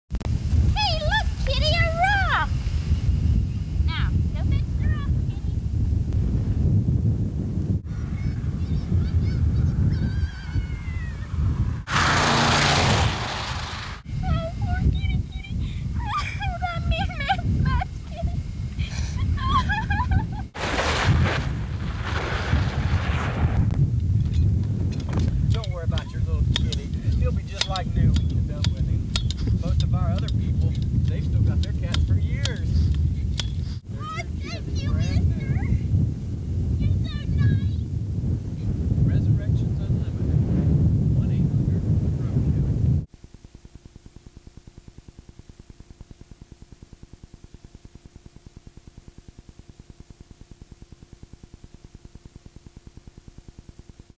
Road Kill Rescue - Richmond Dale SEHS Drama Class Commercial
A commercial made in the spring of 1989 for a high school drama class..

No animals (real or artificial) were harmed in the making of this video.

Re-titled..

Source:  VHS Camcorder Tape copy